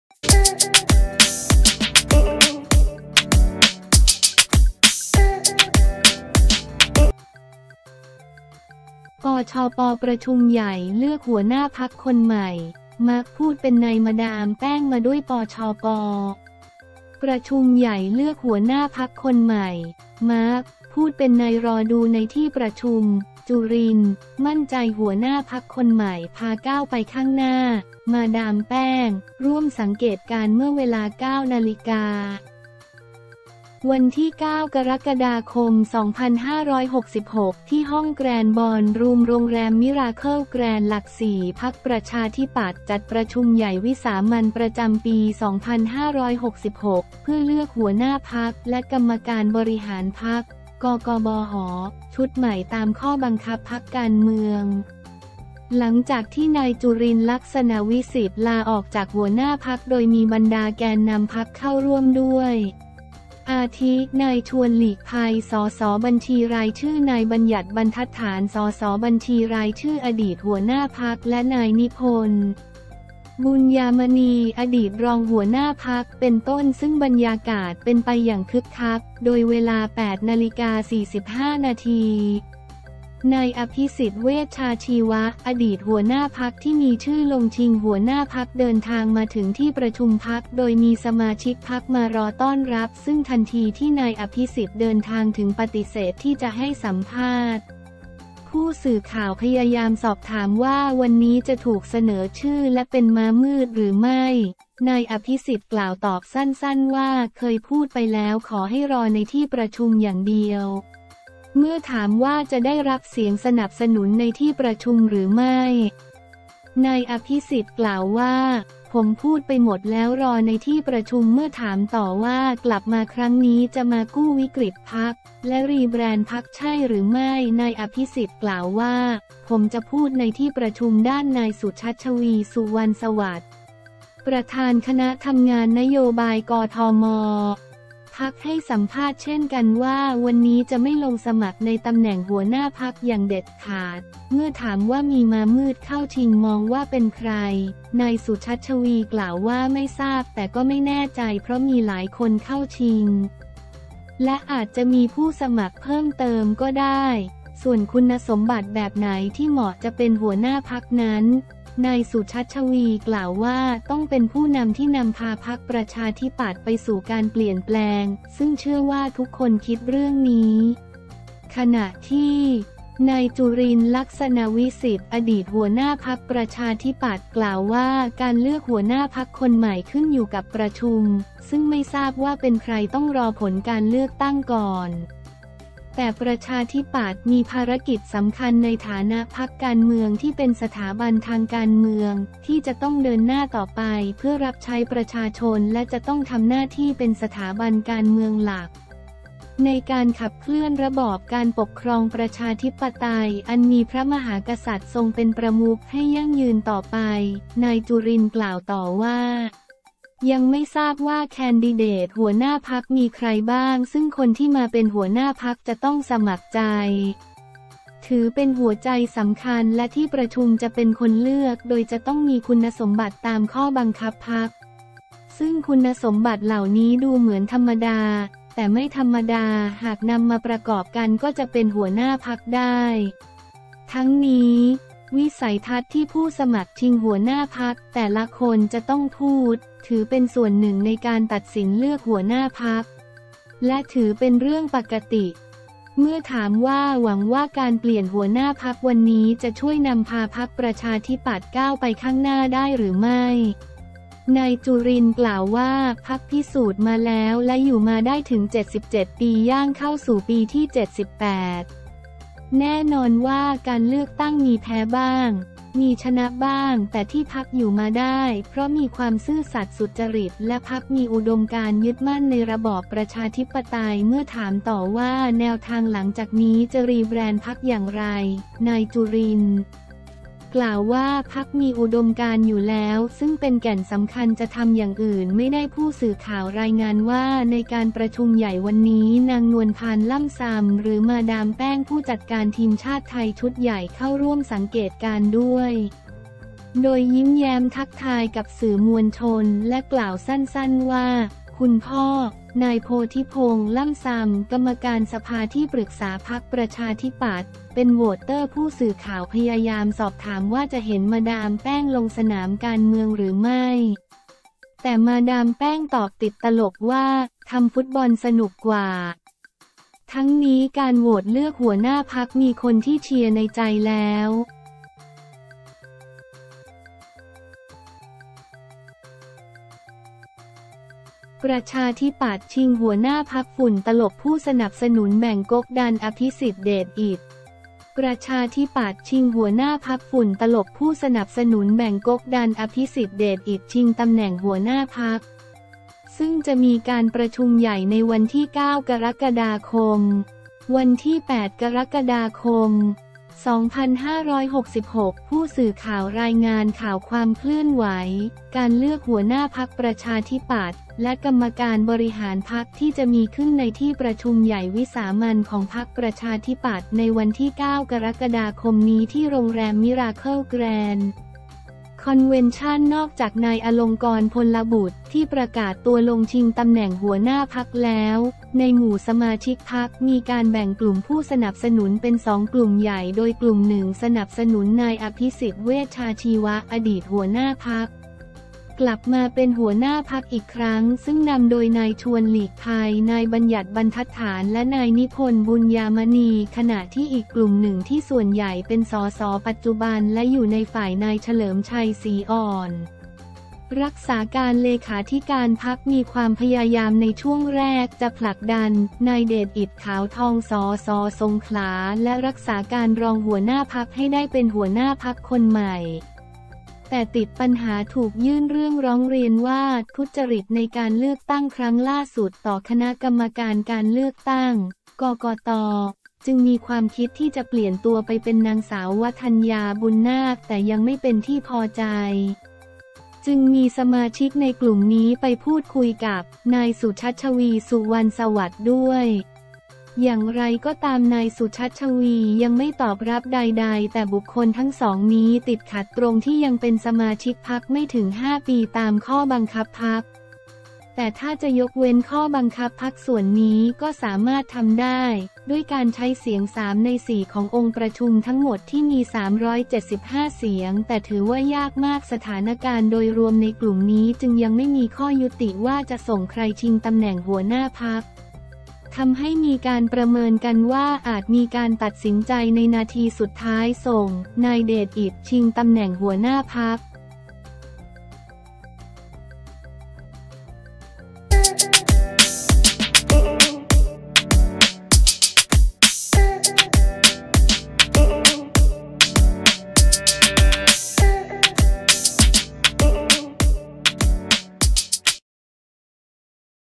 ปชป ประชุมใหญ่เลือกหัวหน้าพรรคคนใหม่ มาร์ค พูดเป็นนัย มาดามแป้ง มาด้วย
ปชป. ประชุมใหญ่เลือกหัวหน้าพรรคคนใหม่ “มาร์ค” พูดเป็นนัยรอดูในที่ประชุม “จุรินทร์” มั่นใจ หน.พรรคคนใหม่พาก้าวไปข้างหน้า “มาดามแป้ง” ร่วมสังเกตการณ์
เมื่อเวลา 09.00 น. วันที่ 9 ก.ค.2566 ที่ห้องแกรนด์บอลรูม โรงแรมมิราเคิล แกรนด์ หลักสี่ พรรคประชาธิปัตย์จัดประชุมใหญ่วิสามัญประจำปี 2566 เพื่อเลือกหัวหน้าพรรคและกรรมการบริหารพรรค (กก.บห.) ชุดใหม่ ตามข้อบังคับพรรคการเมือง หลังจากที่ นายจุรินทร์ ลักษณวิศิษฏ์ ลาออกจากหัวหน้าพรรค โดยมีบรรดาแกนนำพรรคเข้าร่วมด้วย อาทิ นายชวน หลีกภัย ส.ส.บัญชีรายชื่อ นายบัญญัติ บรรทัดฐาน ส.ส.บัญชีรายชื่อ อดีตหัวหน้าพรรค และ นายนิพนธ์ บุญญามณี อดีตรองหัวหน้าพรรคฯเป็นต้น ซึ่งบรรยากาศเป็นไปอย่างคึกคัก

โดยเวลา 08.45 น. นายอภิสิทธิ์ เวชชาชีวะ อดีตหัวหน้าพรรค ที่มีชื่อลงชิงหัวหน้าพรรคฯ เดินทางมาถึงที่ประชุมพรรค โดยมีสมาชิกพรรคมารอต้อนรับ ซึ่งทันทีที่นายอภิสิทธิ์ เดินทางถึงปฏิเสธที่จะให้สัมภาษณ์

ผู้สื่อข่าวพยายามสอบถามว่าวันนี้จะถูกเสนอชื่อและเป็นม้ามืดหรือไม่ นายอภิสิทธิ์ กล่าวตอบสั้นๆว่า เคยพูดไปแล้ว ขอให้รอในที่ประชุมอย่างเดียว
เมื่อถามว่าจะได้รับเสียงสนับสนุนในที่ประชุมหรือไม่ นายอภิสิทธิ์ กล่าวว่า “ผมพูดไปหมดแล้ว รอในที่ประชุม
เมื่อถามต่อว่ากลับมาครั้งนี้จะมากู้วิกฤติพรรค และรีแบรนด์พรรคใช่หรือไม่ นายอภิสิทธิ์ กล่าวว่า “ผมจะพูดในที่ประชุม
ด้าน นายสุชัชวีร์ สุวรรณสวัสดิ์ ประธานคณะทำงานนโยบาย กทม. พรรคฯ ให้สัมภาษณ์เช่นกันว่า วันนี้จะไม่ลงสมัครในตำแหน่งหัวหน้าพรรคฯอย่างเด็ดขาด

เมื่อถามว่ามีมามืดเข้าชิงมองว่าเป็นใคร นายสุชัชวีร์ กล่าวว่า ไม่ทราบ แต่ก็ไม่แน่ใจ เพราะมีหลายคนเข้าชิง และอาจจะมีผู้สมัครเพิ่มเติมก็ได้

ส่วนคุณสมบัติแบบไหนที่เหมาะจะเป็นหัวหน้าพรรคนั้น นายสุชัชวีร์ กล่าวว่า ต้องเป็นผู้นำ ที่นำพาพรรคประชาธิปัตย์ไปสู่การเปลี่ยนแปลง ซึ่งเชื่อว่าทุกคนคิดเรื่องนี้
ขณะที่ นายจุรินทร์ ลักษณวิศิษฏ์ อดีตหัวหน้าพรรคประชาธิปัตย์ กล่าวว่า การเลือกหัวหน้าพรรคคนใหม่ขึ้นอยู่กับืี่ประชุม ซึ่งไม่ทราบว่าเป็นใคร ต้องรอผลการเลือกตั้งก่อน แต่ประชาธิปัตย์มีภารกิจสำคัญในฐานะพรรคการเมืองที่เป็นสถาบันทางการเมือง ที่จะต้องเดินหน้าต่อไป เพื่อรับใช้ประชาชน และจะต้องทำหน้าที่เป็นสถาบันการเมืองหลัก ในการขับเคลื่อนระบอบการปกครองประชาธิปไตยอันมีพระมหากษัตริย์ทรงเป็นประมุขให้ยั่งยืนต่อไป

นายจุรินทร์ กล่าวต่อว่า ยังไม่ทราบว่า แคนดิเดตหัวหน้าพรรคมีใครบ้าง ซึ่งคนที่มาเป็นหัวหน้าพรรคจะต้องสมัครใจ ถือเป็นหัวใจสำคัญ และที่ประชุมจะเป็นคนเลือก โดยจะต้องมีคุณสมบัติตามข้อบังคับพรรค ซึ่งคุณสมบัติเหล่านี้ดูเหมือนธรรมดา แต่ไม่ธรรมดา หากนำมาประกอบกันก็จะเป็นหัวหน้าพรรคได้ ทั้งนี้ วิสัยทัศน์ที่ผู้สมัครชิงหัวหน้าพรรคแต่ละคนจะต้องพูด ถือเป็นส่วนหนึ่งในการตัดสินเลือกหัวหน้าพรรค และถือเป็นเรื่องปกติ

เมื่อถามว่าหวังว่าการเปลี่ยนหัวหน้าพรรควันนี้จะช่วยนำพาพรรคประชาธิปัตย์ก้าวไปข้างหน้าได้หรือไม่ นายจุรินทร์ กล่าวว่า พรรคฯพิสูจน์มาแล้ว และอยู่มาได้ถึง 77 ปี ย่างเข้าสู่ปีที่ 78 แน่นอนว่าการเลือกตั้งมีแพ้บ้าง มีชนะบ้าง แต่ที่พรรคอยู่มาได้ เพราะมีความซื่อสัตย์สุจริต และพรรคมีอุดมการณ์ ยึดมั่นในระบอบประชาธิปไตย
เมื่อถามต่อว่าแนวทางหลังจากนี้จะรีแบรนด์พรรคอย่างไร นายจุรินทร์ กล่าวว่า พรรคมีอุดมการณ์อยู่แล้ว ซึ่งเป็นแก่นสำคัญ จะทำอย่างอื่นไม่ได้

ผู้สื่อข่าวรายงานว่า ในการประชุมใหญ่ฯวันนี้ นางนวลพรรณ ล่ำซำ หรือ มาดามแป้ง ผู้จัดการทีมชาติไทยชุดใหญ่ เข้าร่วมสังเกตการณ์ด้วย โดยยิ้มแย้มทักทายกับสื่อมวลชนและกล่าวสั้นๆว่า “คุณพ่อ (นายโพธิพงษ์ ล่ำซำ กรรมการสภาที่ปรึกษาพรรคประชาธิปัตย์) เป็นโหวตเตอร์
ผู้สื่อข่าวพยายามสอบถามว่าจะเห็นมาดามแป้งลงสนามการเมืองหรือไม่ แต่มาดามแป้งตอบติดตลกว่า “ทำฟุตบอลสนุกกว่า” ทั้งนี้ การโหวตเลือกหัวหน้าพรรค มีคนที่เชียร์ในใจแล้ว